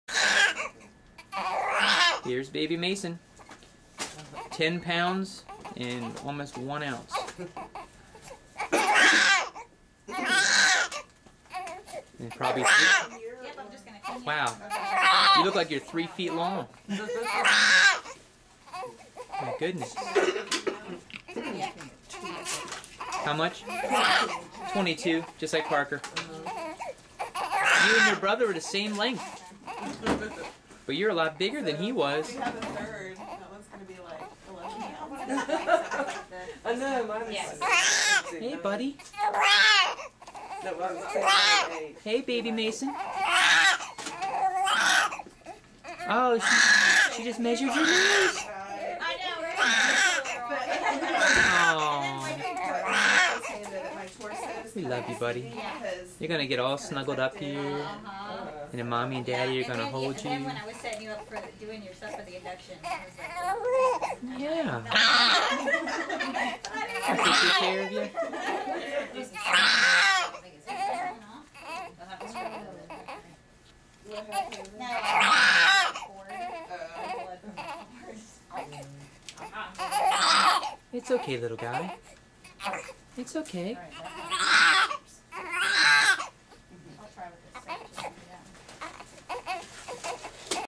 Our son Mason right after being born.
Here is mason getting measured right after being born.  He was 10 lbs 1 oz and 22 inches long.  What a big boy!